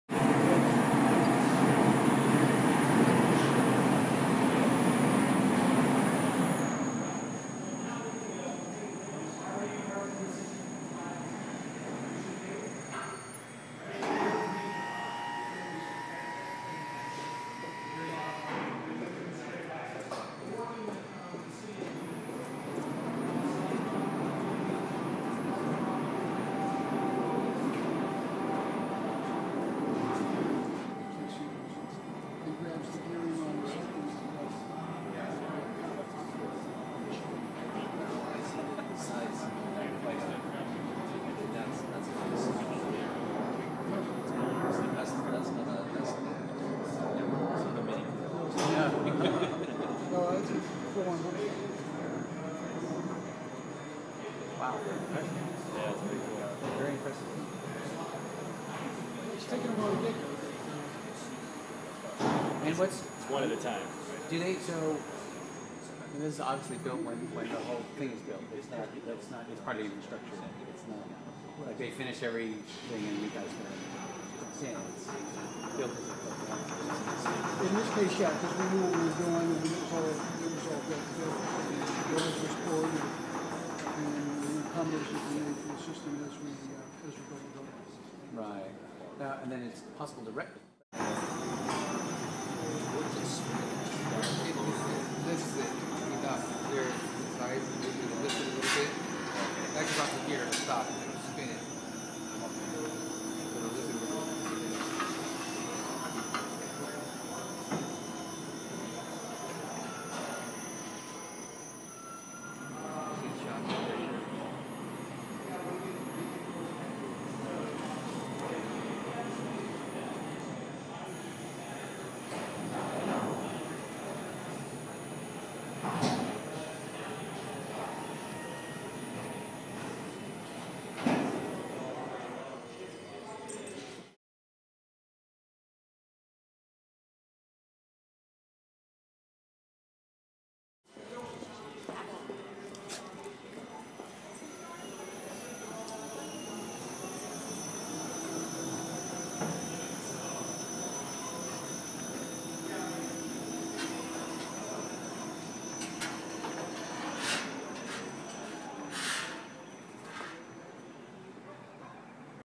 Residential Automated Parking Lot Debuts in Los Angeles
Good news for Los Angeles residents looking for a parking spot after a grueling commute; the first of its kind automated parking system broke ground this month in Van Nuys, CA. The 16 car capacity system brings an industrial (and compact) approach to residential auto parking and sports a modern, simple user interface to make parking and retrieving your car a snap.